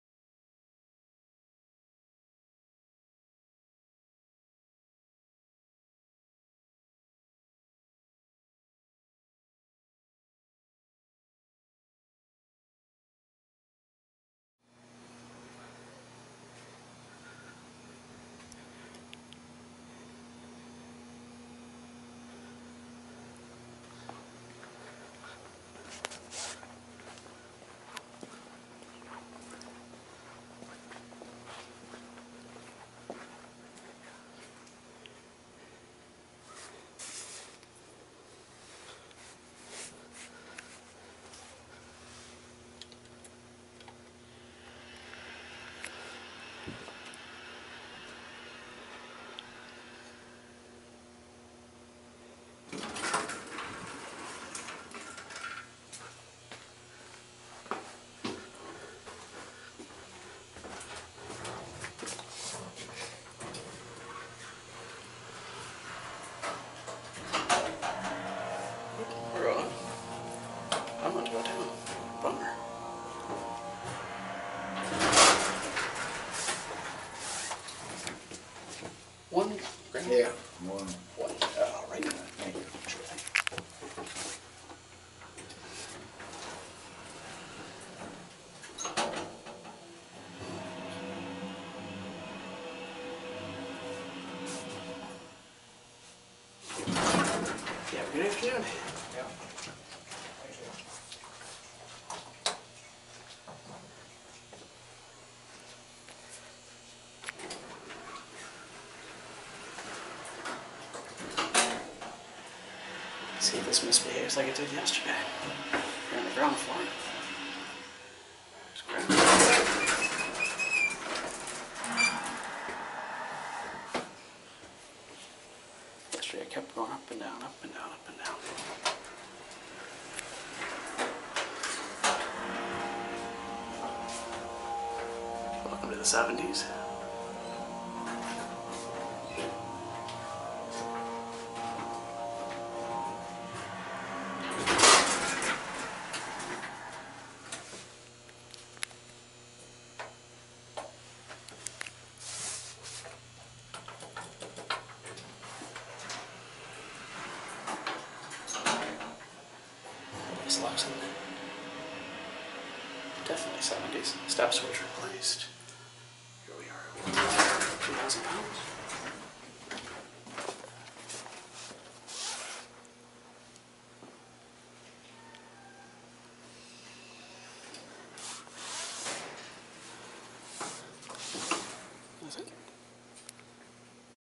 A revisit of the 1970's OTIS hydraulic elevator @ the Fairport Village Landing.
Does it still have the releveling issue?